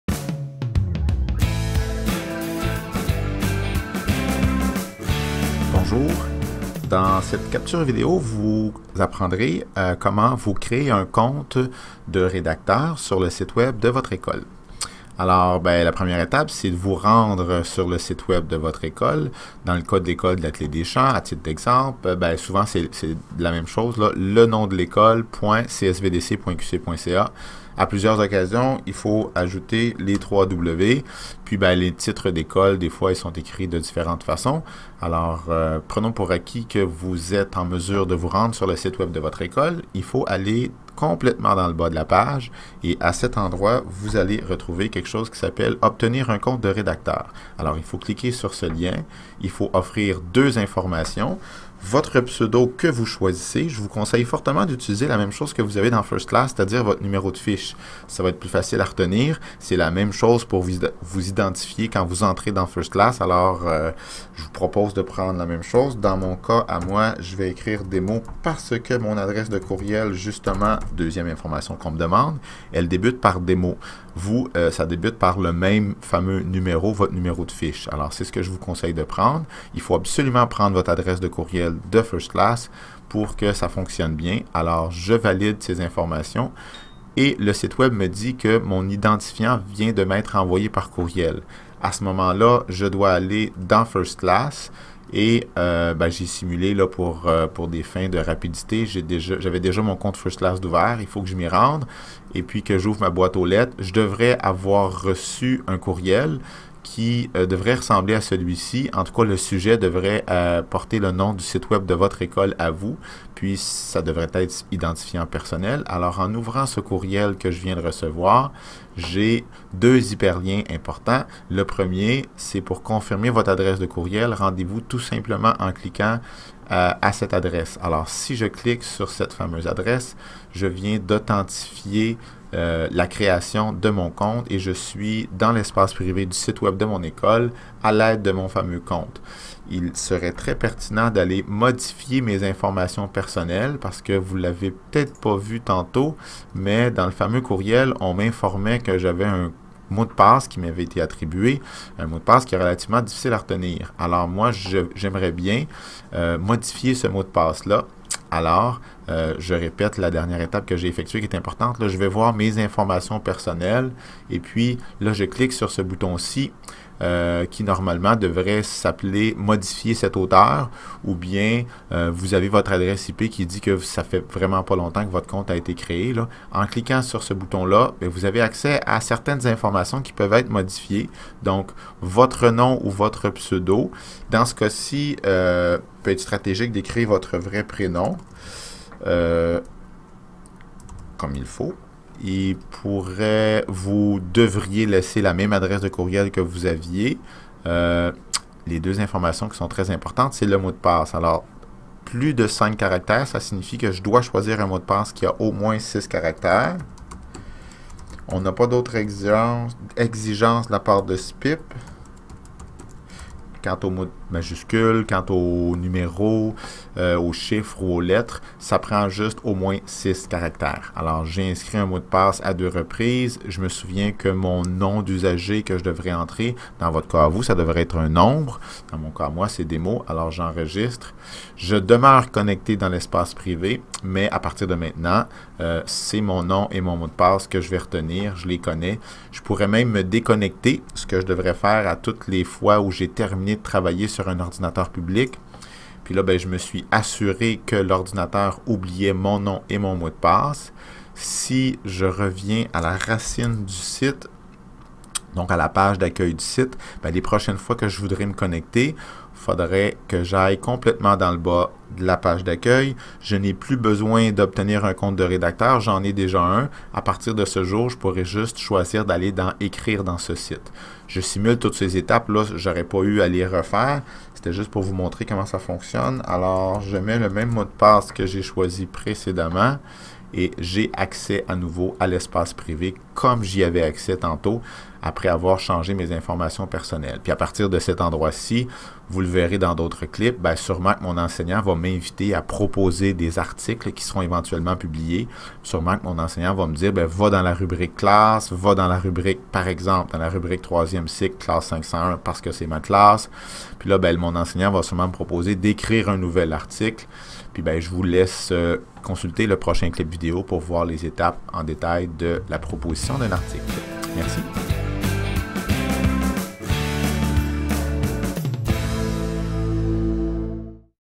Creation d'un compte de redacteur SPIP 3
Étapes pour permettre aux élèves de se créer un compte eux-mêmes sur les sites des écoles de la CSVDC. À l'aide de ce compte, ils pourront proposer des articles que vous pourrez publier par la suite.